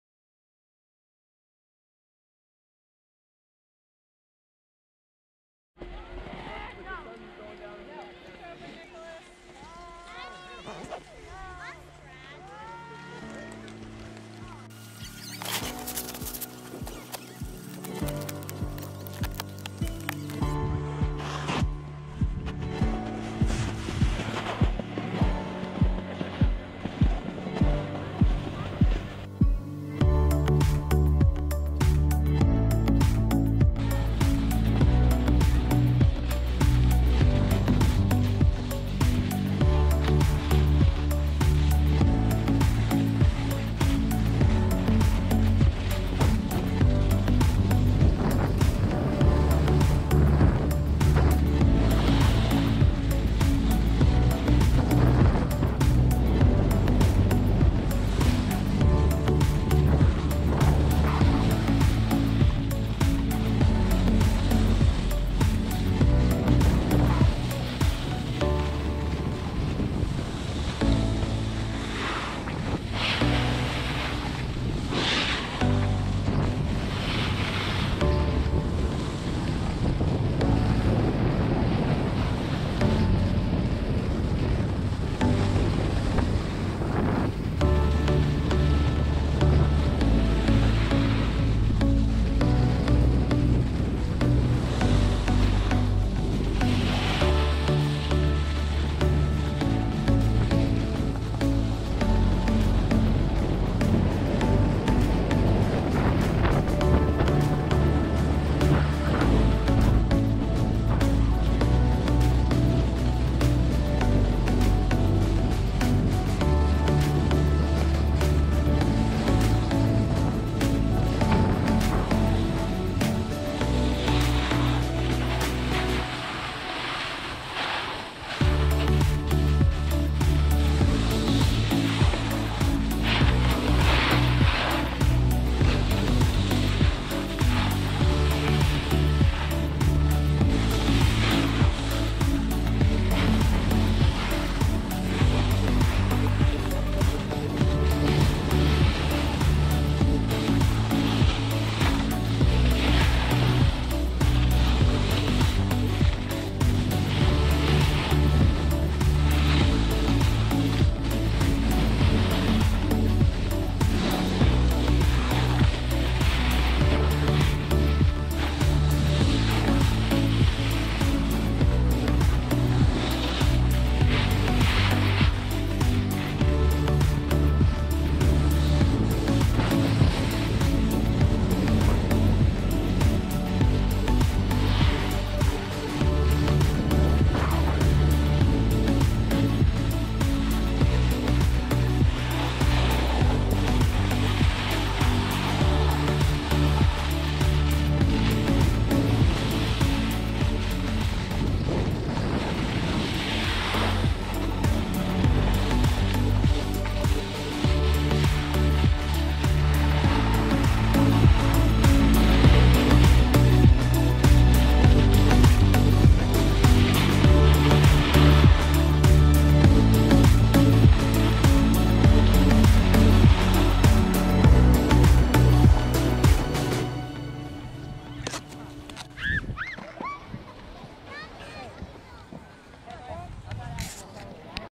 Wisp Ski Resort Muskrat & Boulder Trails [Early Season 2019]
A run down the Muskrat and Boulder runs at Wisp Ski Resort. Video taken on 12/28/2019 with temps in the low 50s.

I did not receive any compensation from Ski Appalachia for producing this video. Just helping out a friend. :)